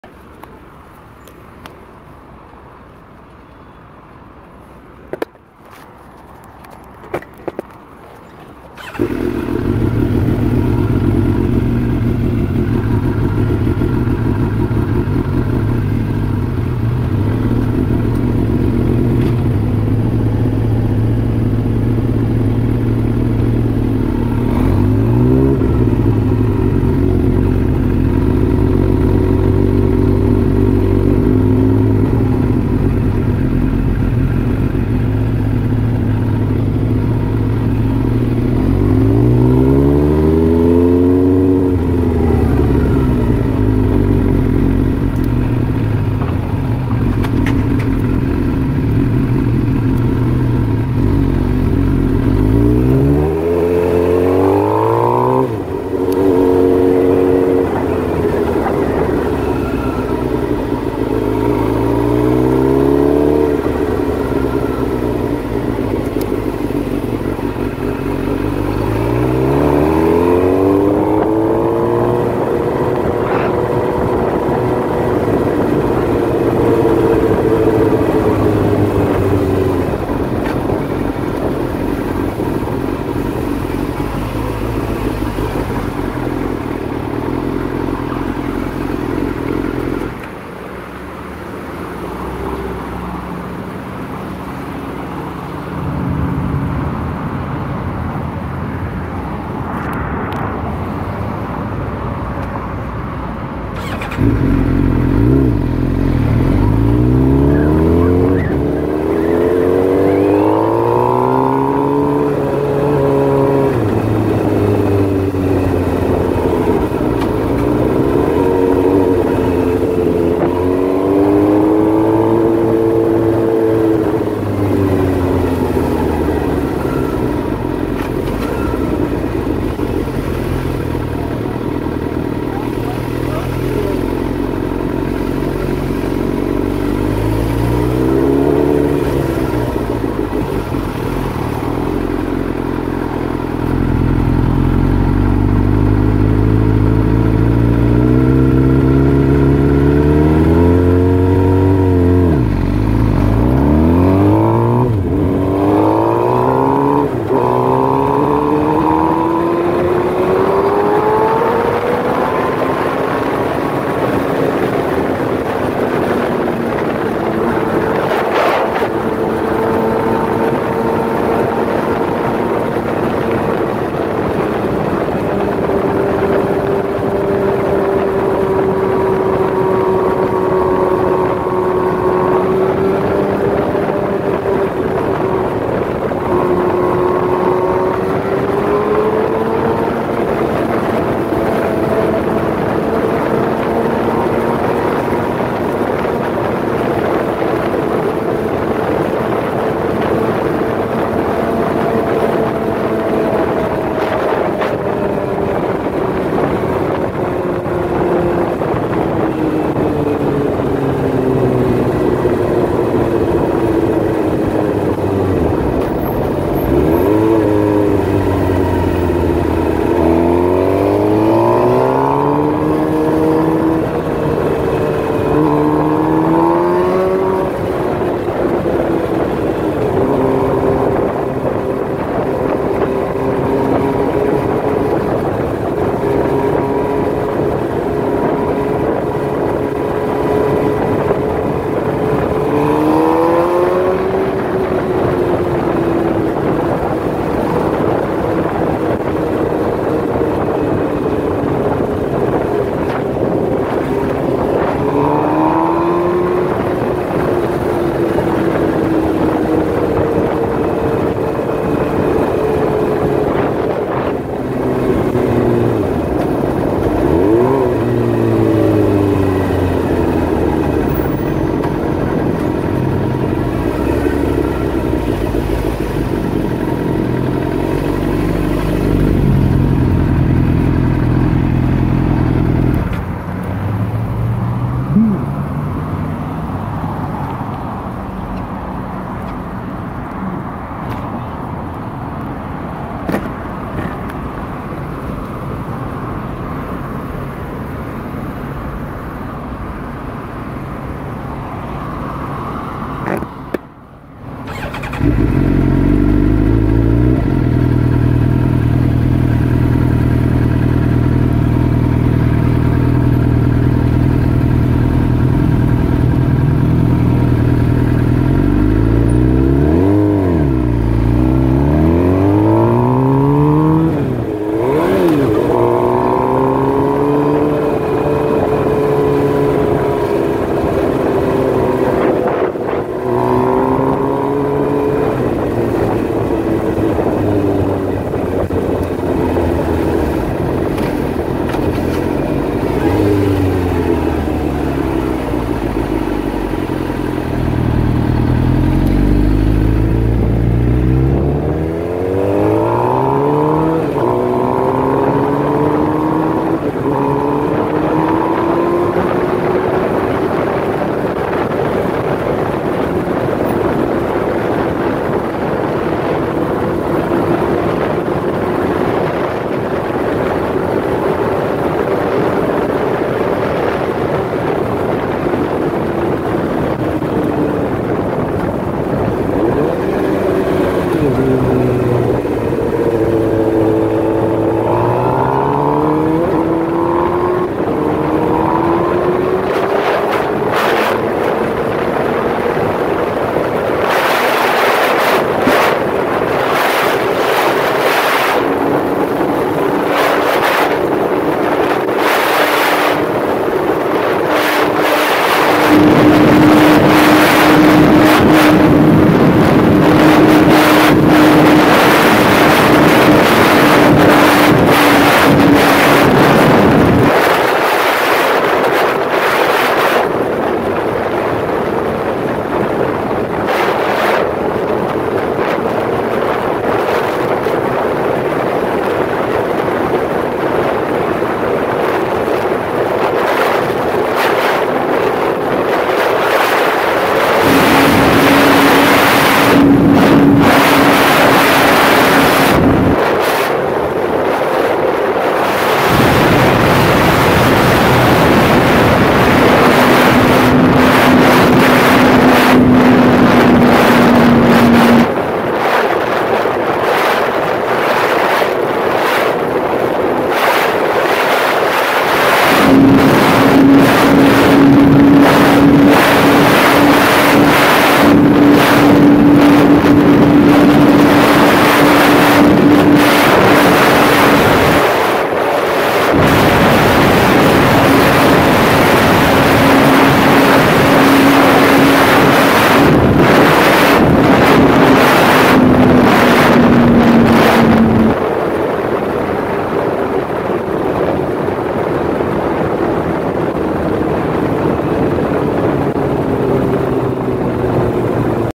Going Fast From Target!
After picking up some things from Target I go down Sepulveda then reach speeds that I never anticipated. Third week on Motorcycle and it is both fun and very scary.